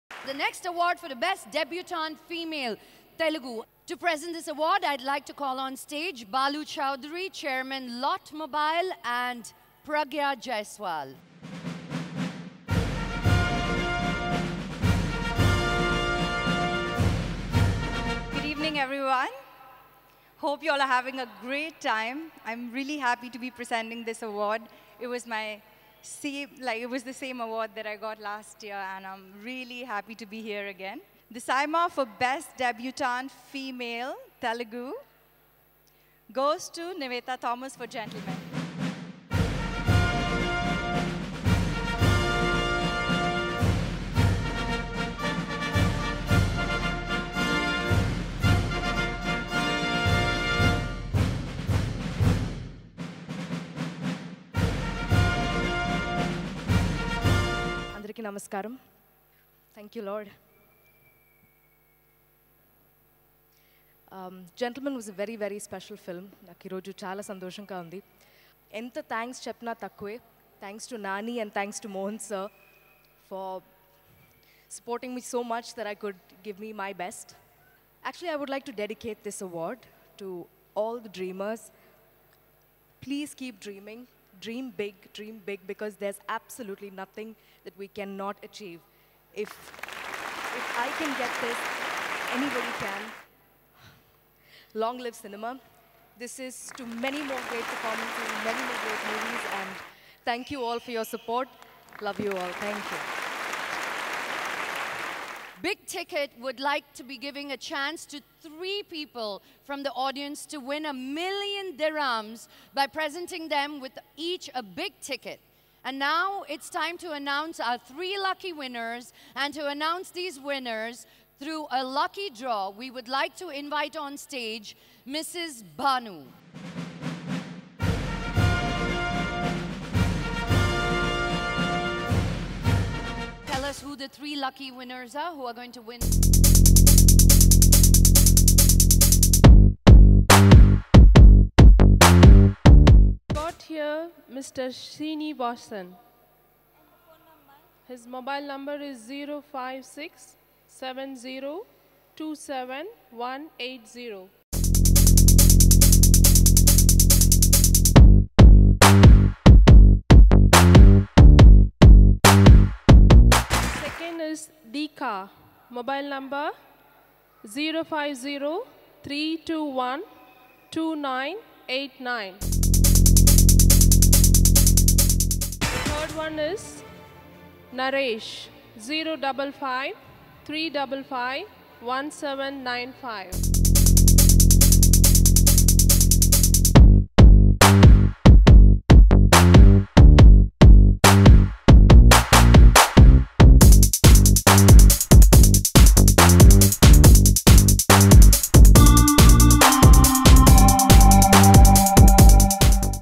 Natural Star Nani Impressed On Nivetha Thomas Cute Speech
Natural Star Nani Impressed On Nivetha Thomas Cute Speech At South Indian Awards Show.